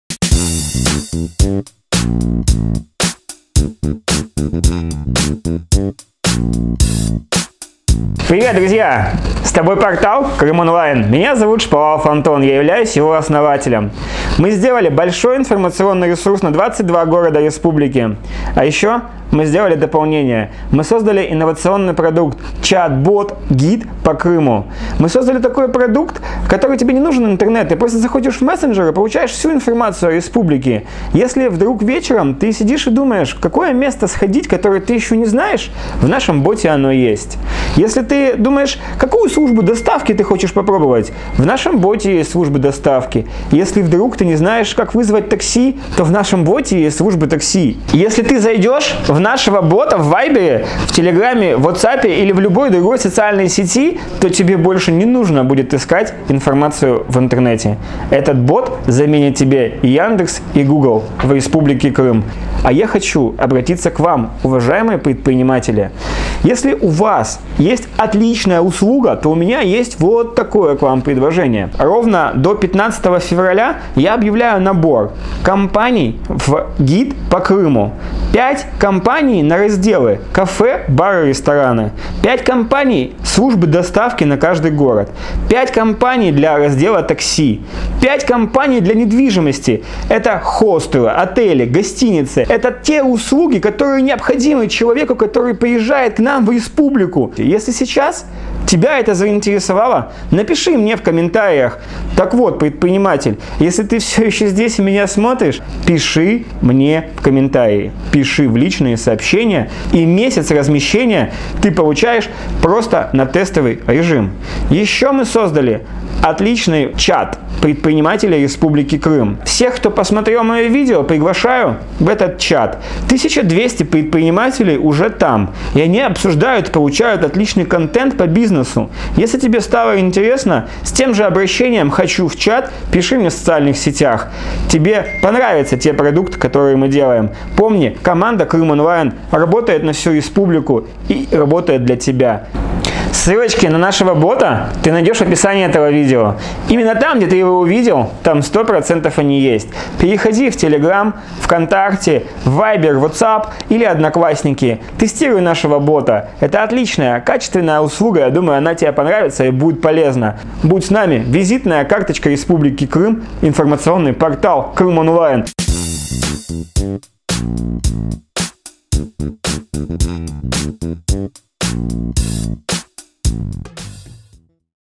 Гид по Крыму в телефоне. Жилье, Развлечения, Отдых, Туры, Достопримечательности Крыма в телефоне.
Команда Крым Онлайн создала полезного Чат Бота "Гид по Крыму". Чат бот который ответит Вам на вопросы : Где покушать?, Куда сходить, что посмотреть?, Где остановиться?, Достопримечательности и экскурсии Крыма.
Приглашаем компании представляющие такие услуги протестировать Бота.
Ссылки на нашего бота

Вайбер: viber://pa?chatURI=crimea-online&context=ml1

Воцап запустим 25 февраля.

Ссылки на Шаповалова Антона